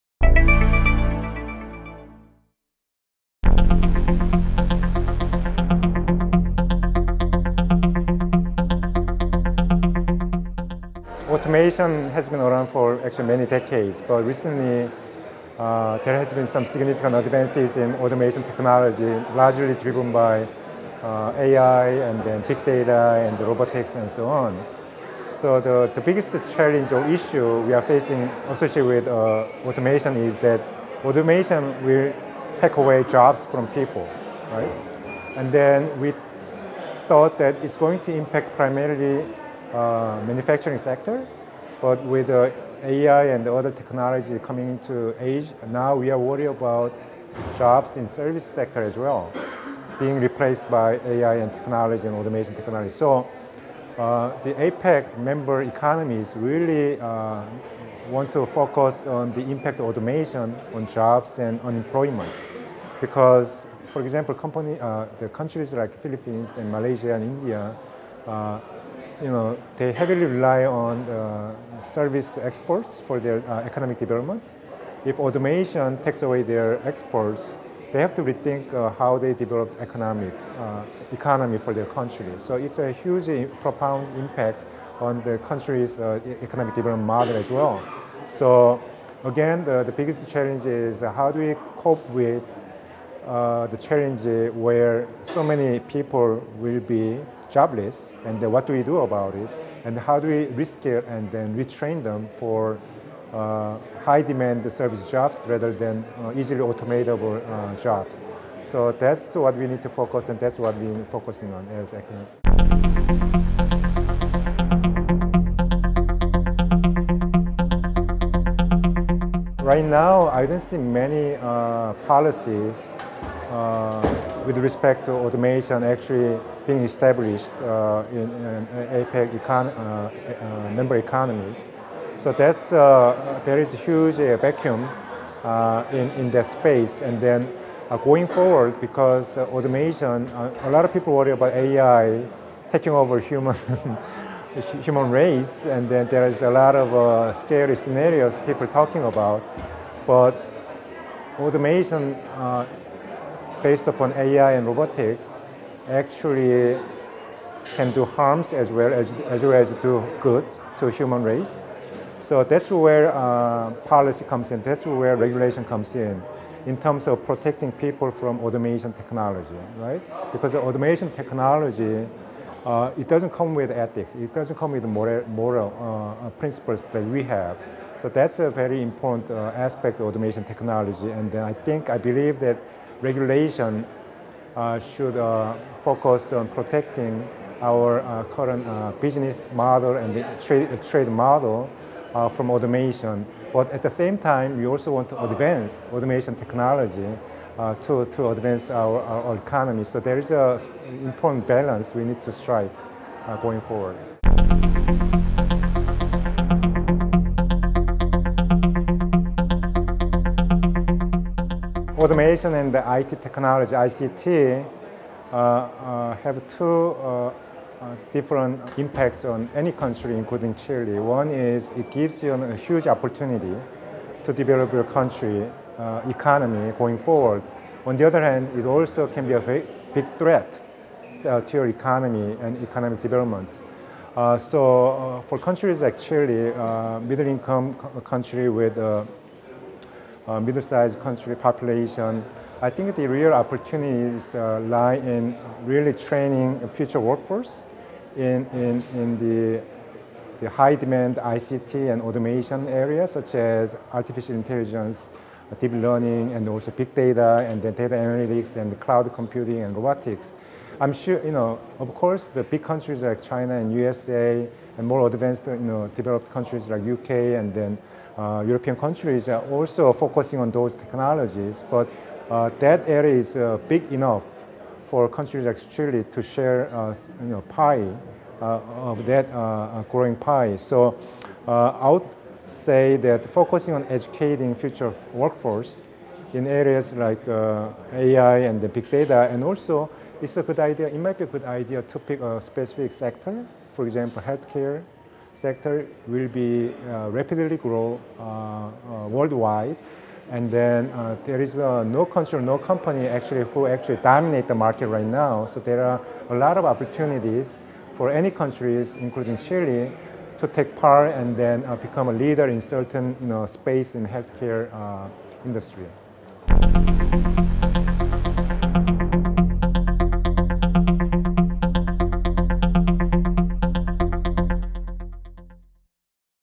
Gwanhoo Lee y la automatización de las economías en el Pacífico
Profesor en tecnologías de la información en la Kogod School of Business de la American University en Estados Unidos, Gwanhoo Lee respaldó el desarrollo de la automatización en conjunto con la Inteligencia Artificial y la robótica en las economías de la Cuenca del Pacífico. Más detalles de esta entrevista en la nota publicada por el Programa Asia Pacífico de la BCN